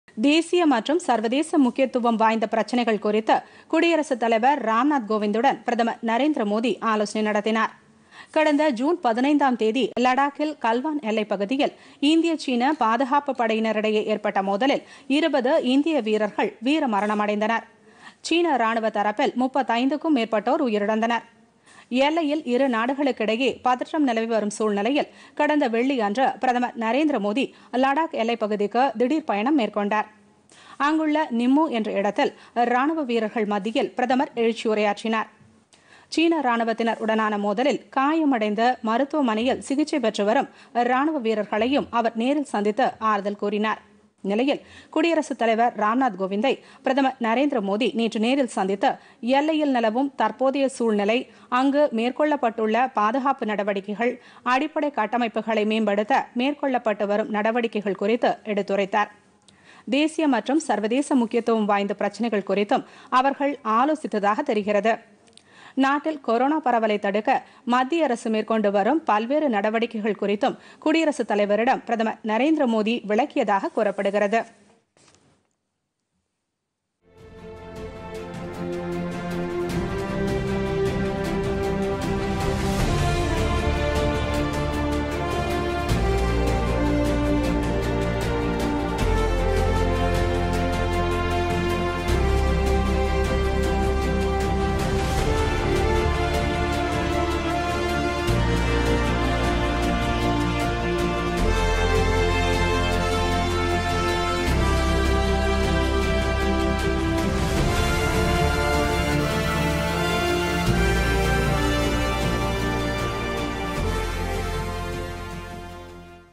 PodhigaiTamilNews பொதிகைசெய்திகள்
குடியரசுத் தலைவர் ராம்நாத் கோவிந்துடன் பிரதமர் நரேந்திர மோதி சந்திப்பு - தேசிய, சர்வதேச முக்கியத்துவம் வாய்ந்த பிரச்சினைகள் குறித்து ஆலோசனை.Visit : TamilNews - Doordarshan